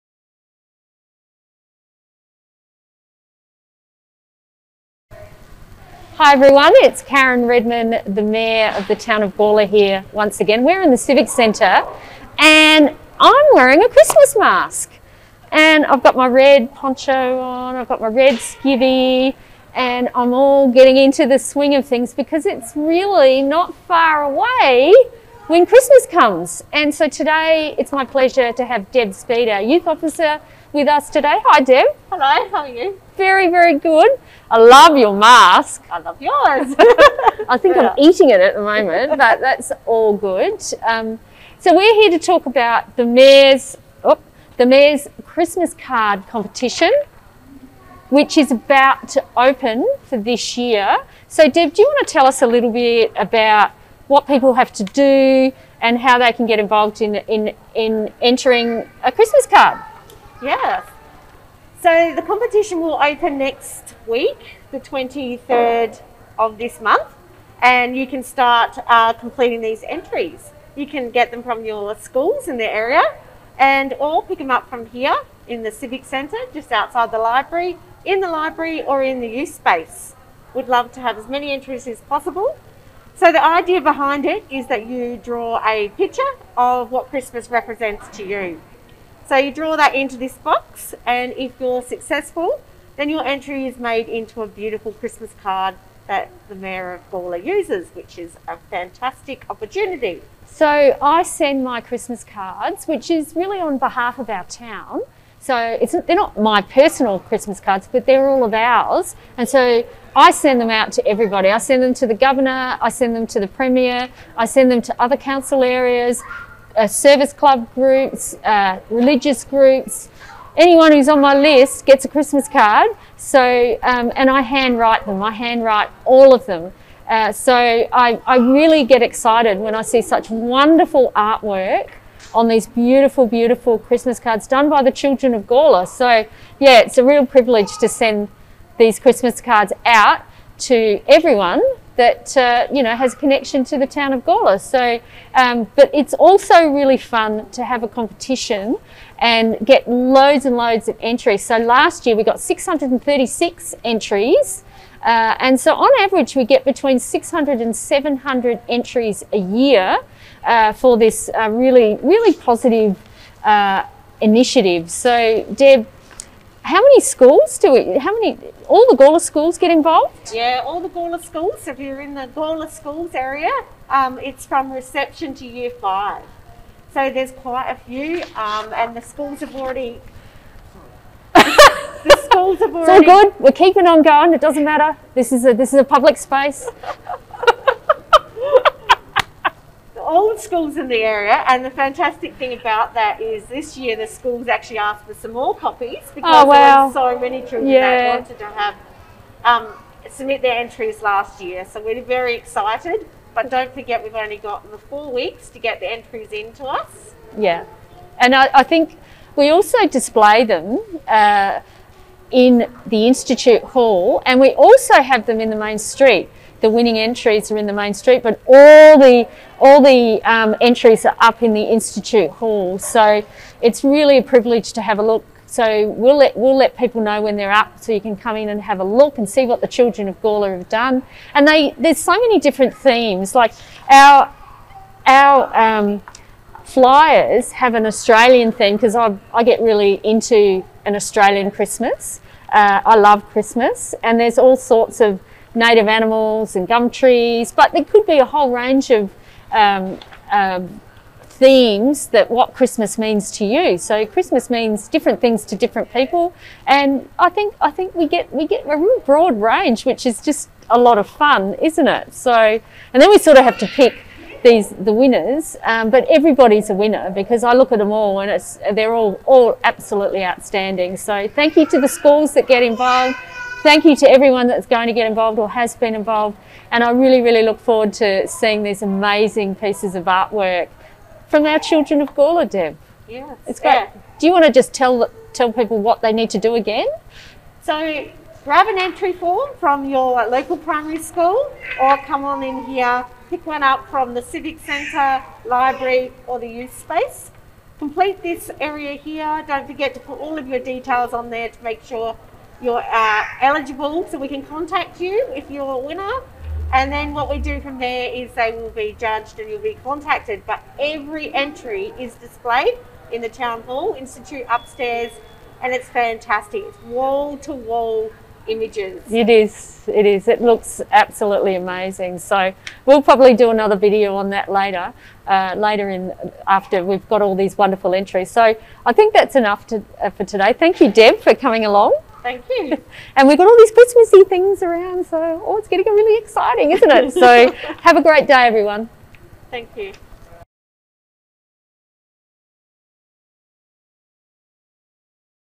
18 August 2021 - Mayors Weekly video
Mayor Karen Redman discusses the Mayors Christmas Card Competition with Debbie Speed, Council's Youth Development Officer.